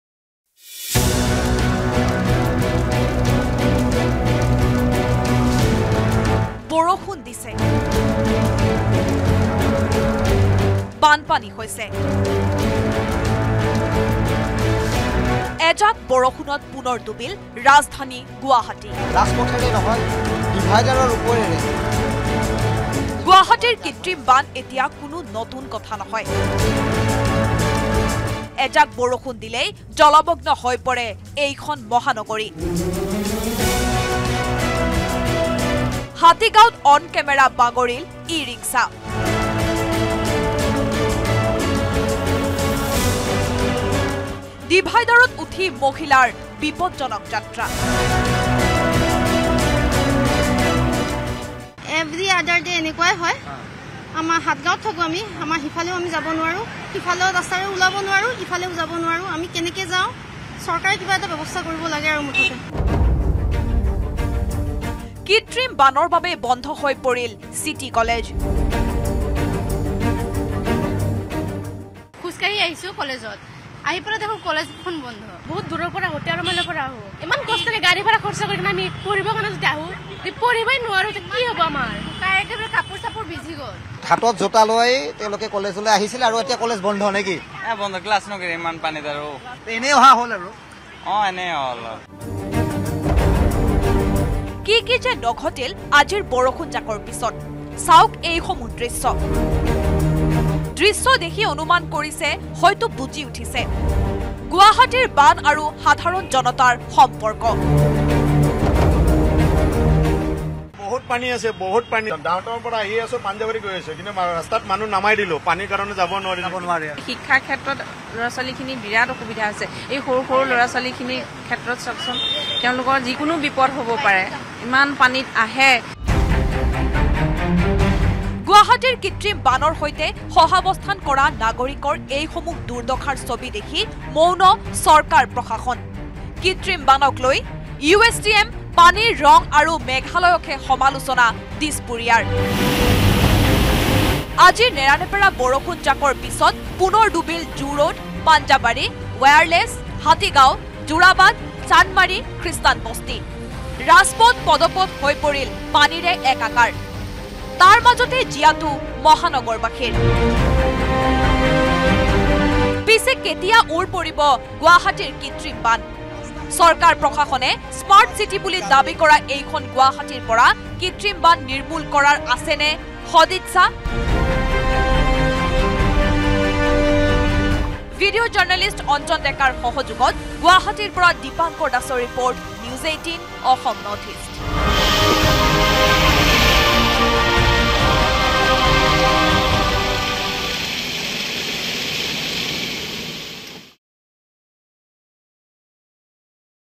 Guwahati Flood : এজাক বৰষুণত পুনৰ জলমগ্ন মহানগৰীৰ বিভিন্ন অঞ্চল | N18V
এজাক বৰষুণত পুনৰ জলমগ্ন মহানগৰীৰ বিভিন্ন অঞ্চল। হাতীগাঁও, ৰুক্মিণীগাঁও, পাঞ্জাবাৰী, বেলতলা, ছয়মাইল, চানমাৰি, যোৰাবাটকে ধৰি মহানগৰীৰ বহু অঞ্চল জলমগ্ন। হাতীগাঁৱত ৰাজপথত পানীৰ মাজত বাগৰি পৰিল ই-ৰিক্সা। সন্ধিয়ালৈকে হাতীগাঁৱৰ পথত কমা নাই পানী।